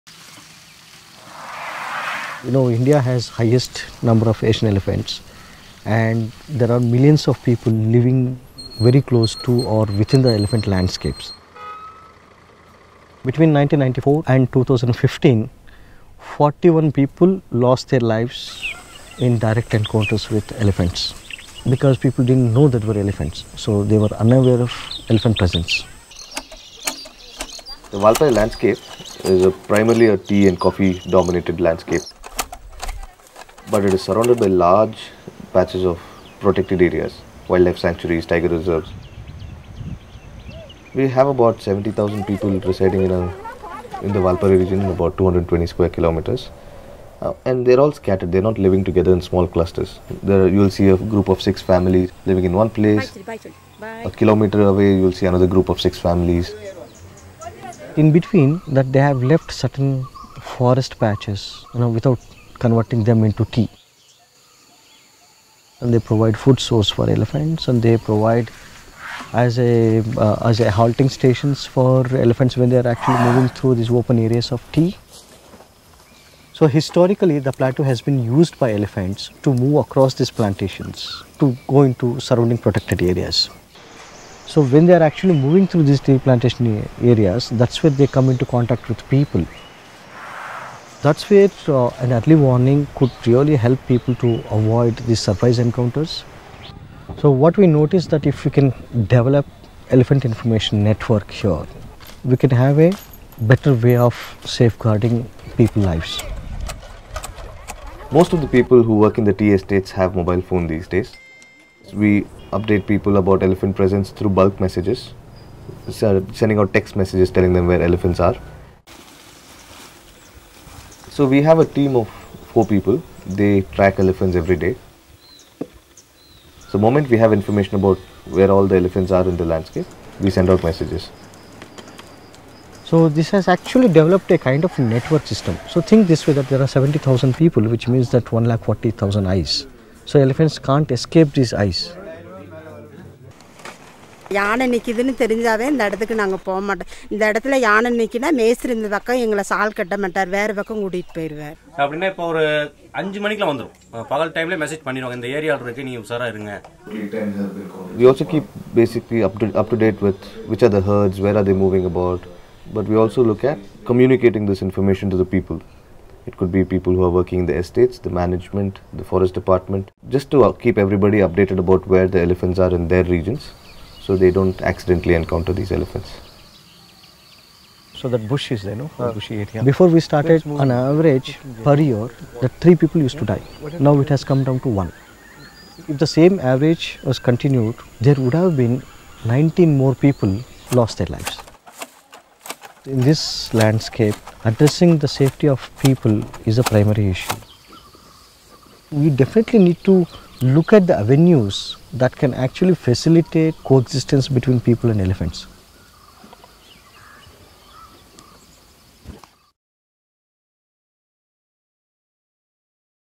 Text Messaging Helps Elephants and People Coexist | National Geographic
Technology is helping protect people from roaming elephants. Cell phones are ubiquitous in rural Valparai, India. And so are Asian elephants. Now the former are being used to keep people safe from the latter. Thousands of people in Valparai work on tea and coffee plantations that are nestled between protected areas—and act as elephant passageways. Since 1994, more than 40 people in the area have been killed in encounters with elephants. Research has shown that most of those deaths could have been prevented if people had had sufficient warning. That’s where the Nature Conservation Foundation’s early warning system comes in. The system began as a crawl on the local cable TV station and now also includes red indicator beacons and bulk text messages, alerting people who live and work in the area to the presence of elephants. The death rate from elephant encounters has dropped since the advent of the warning system, helping people and elephants coexist.

PRODUCED AND FILMED BY: Varun M Nayer
ADDITIONAL FOOTAGE: Ganesh Raghunsthan and the Nature Conservation Foundation